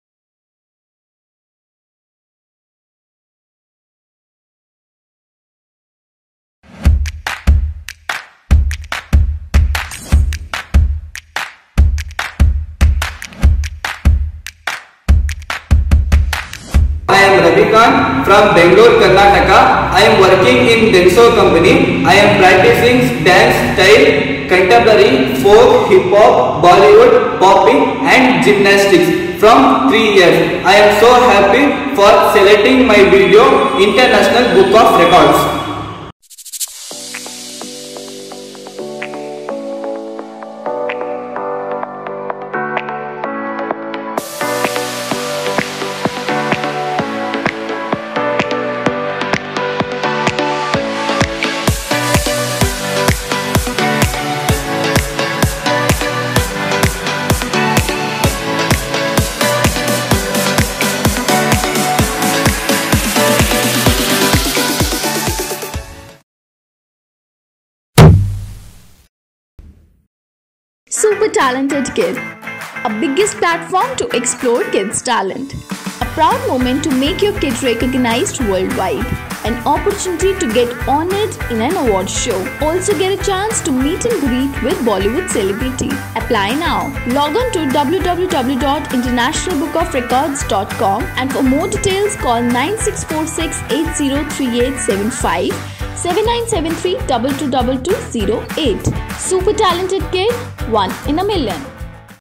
MOST KICK UPS IN 30 SECONDS
The World Record of “MOST KICK UPS IN 30 SECONDS” is achieved by RABI KHAN on 7th June 2021 from Bangalore (Karnataka) India. He did 23 kick ups in 30 seconds and has set a new world record for International Book of Records.